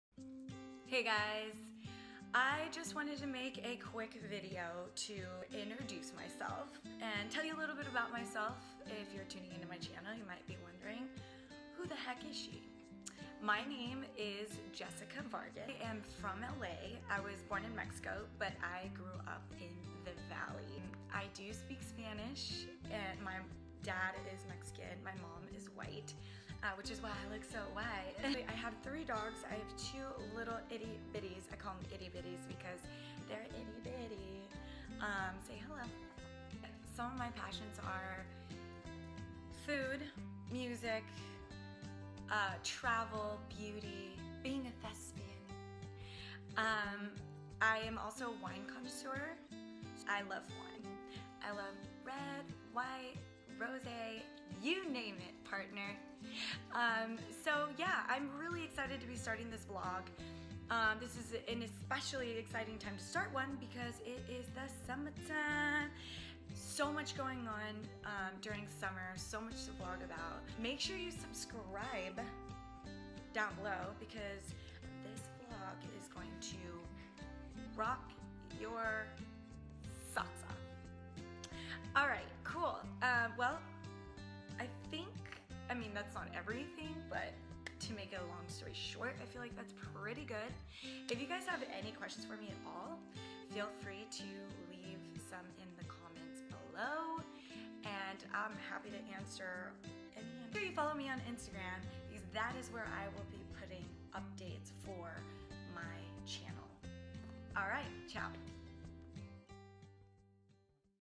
Jess Goes West intro
Hey y'all! Here is my first video introducing myself.